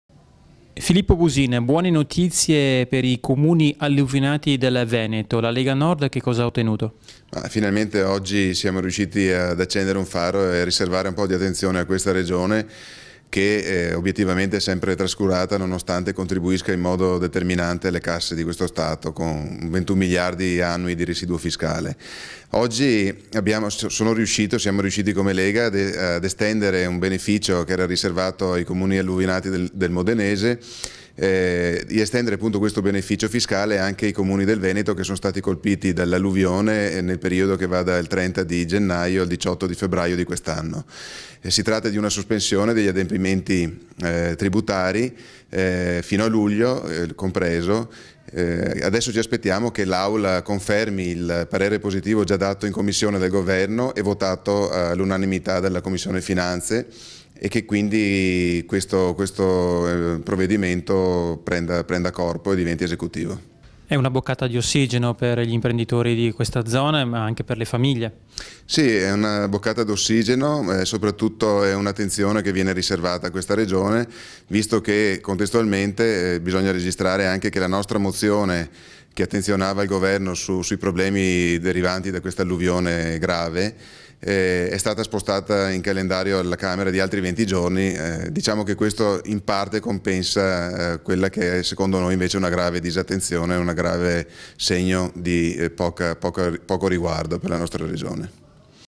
BUSIN (LN), SOSPESO PAGAMENTO TRIBUTI ANCHE PER COMUNI VENETI ALLUVIONATI
ROMA, 13 MAR - "Grazie alla Lega Nord e' stato
ottenuto un grande risultato a favore dei comuni veneti colpiti
tra gennaio e febbraio dai violenti alluvioni. Sono infatti
stati estesi anche ai comuni del Veneto, con parere favorevole
del governo e della commissione Finanze, i benefici gia' previsti
per i comuni del modenese nel decreto per il rientro dei
capitali. Il provvedimento prevede la sospensione dei tributi
per il periodo compreso tra il 17 gennaio e il 31 luglio 2014.
Adesso ci attendiamo il voto favorevole dell'Aula per una
battaglia di equita' e giustizia nei confronti di una regione che
ogni anno regala 21 miliardi allo Stato". Lo dichiara il
capogruppo della Lega Nord in commissione Finanze, Filippo
Busin, presentatore dell'emendamento.